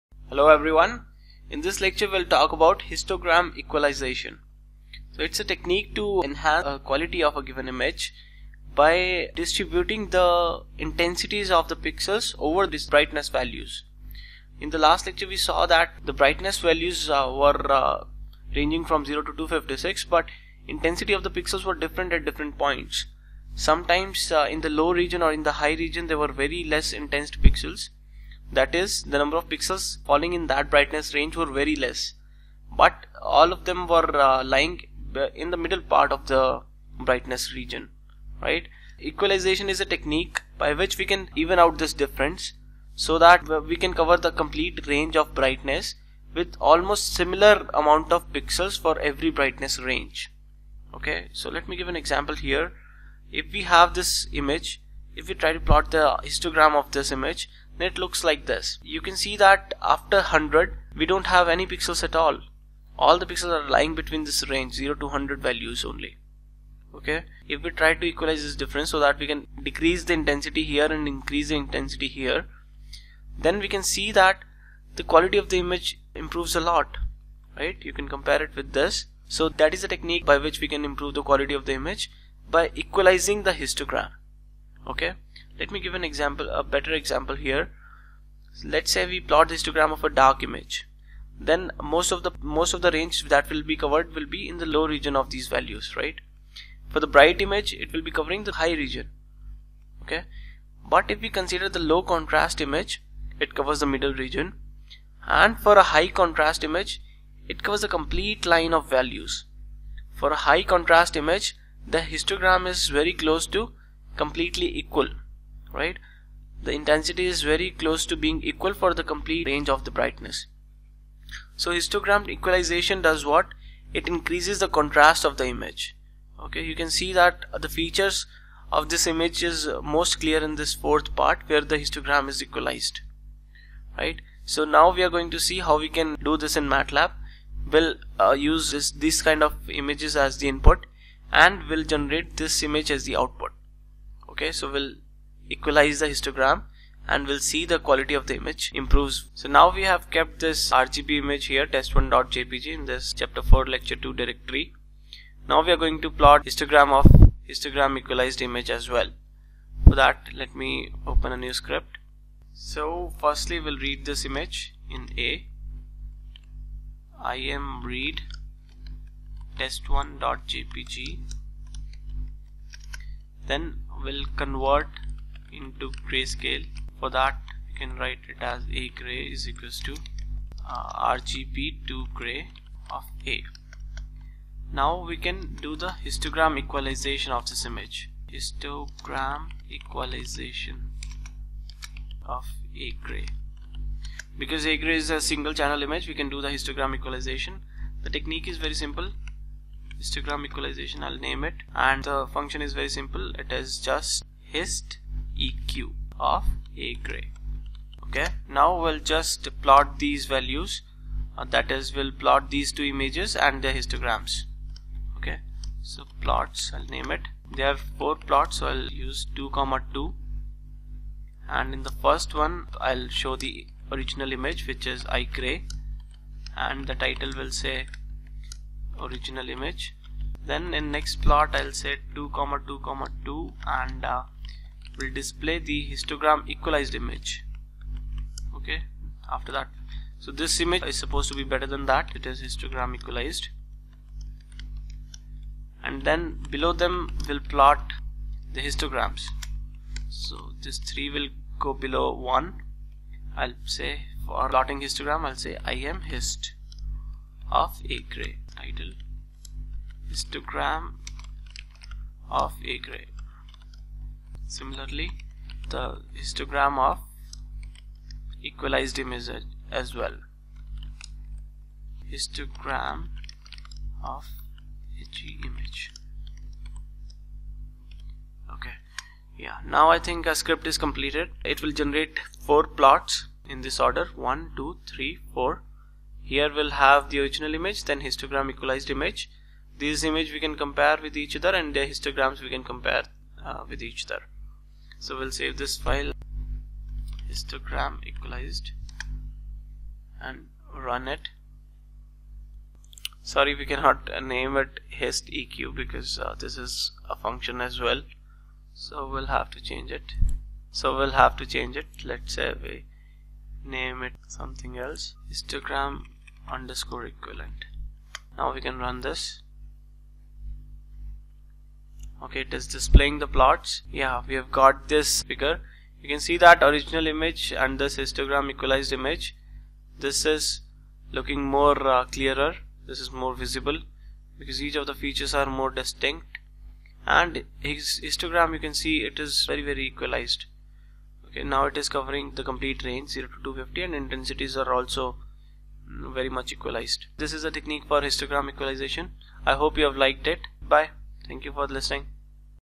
Histogram Equalization - Lec 2 of 3
Learn about histogram equalization and how to do it in MATLAB with examples.

For complete course on image processing using MATLAB, join my course at a amazing discount of 90%